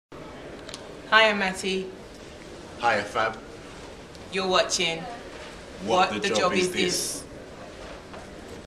Whathejobisthis teaser
Helping people who are experiencing a career transition to get better perspective on what's next in their lives because stories are more powerful, than theories.